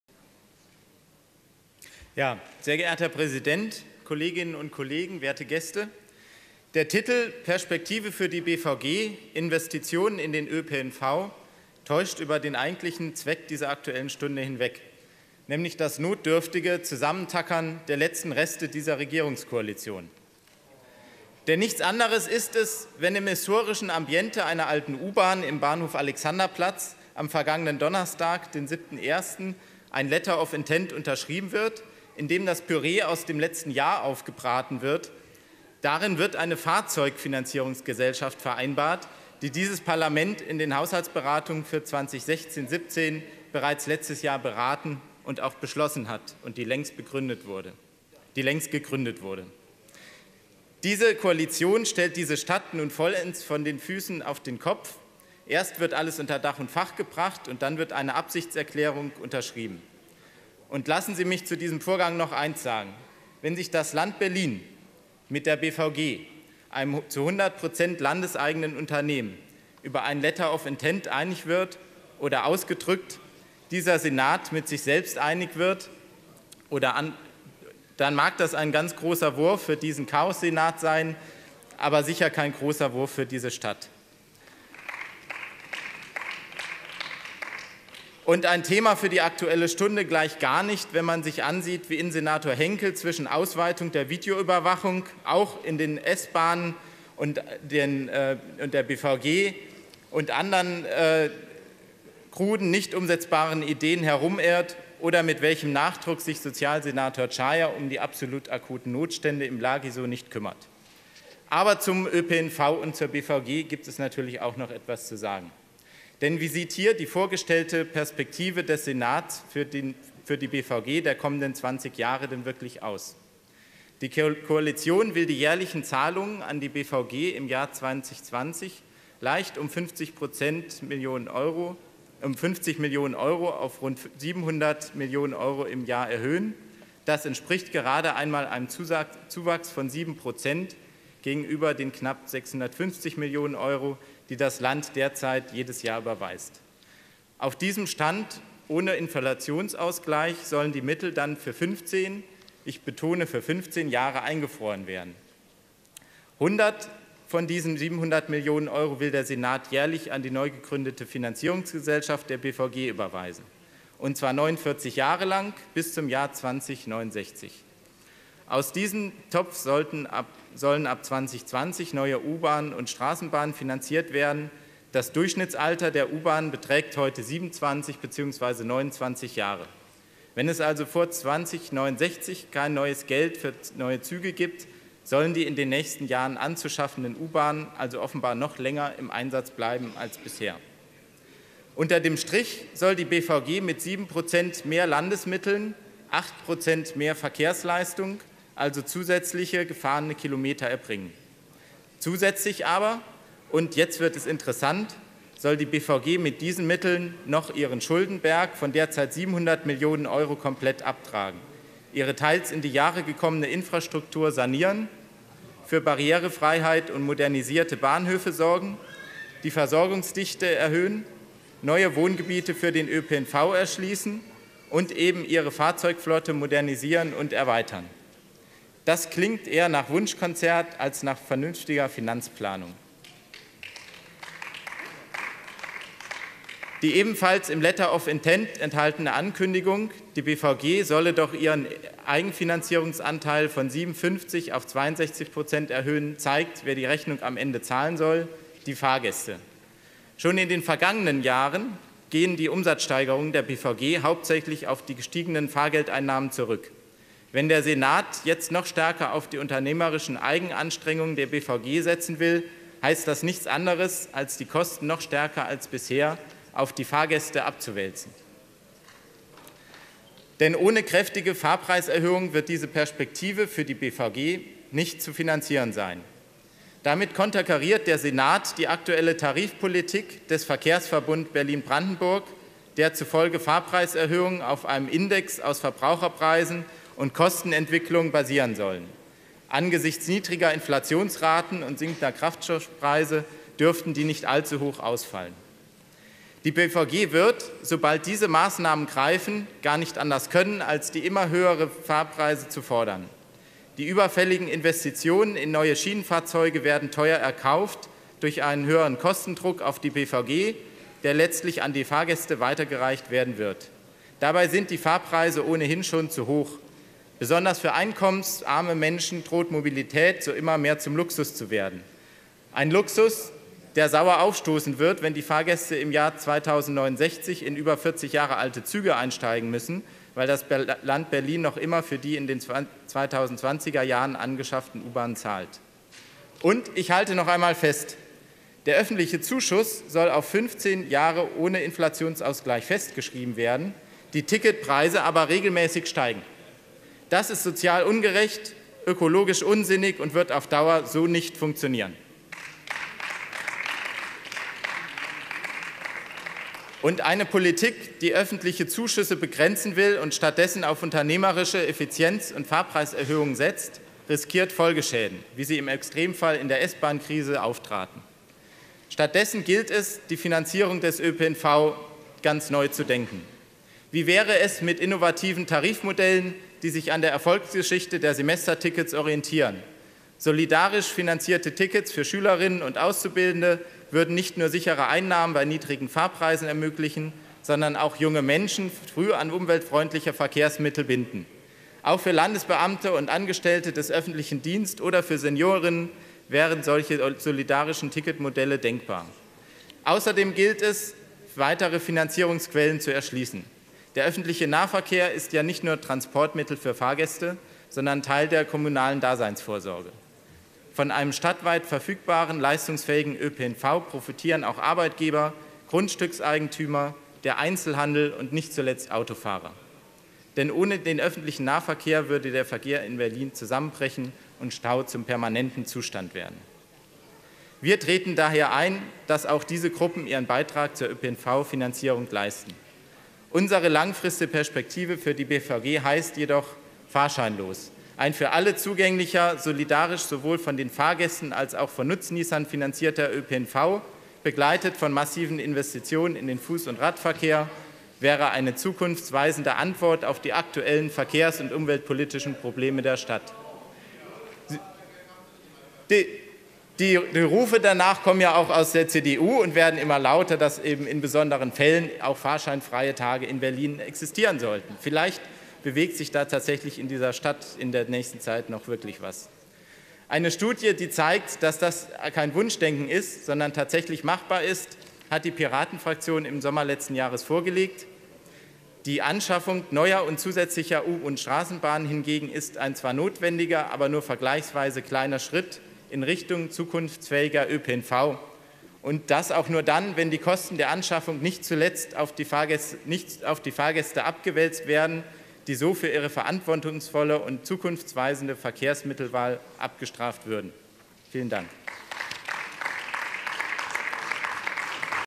Investition in den ÖPNV - Andreas Baum - 74. Plenum
74. Plenarsitzung des Berliner Abgeordnetenhauses am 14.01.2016
Redner: Andreas Baum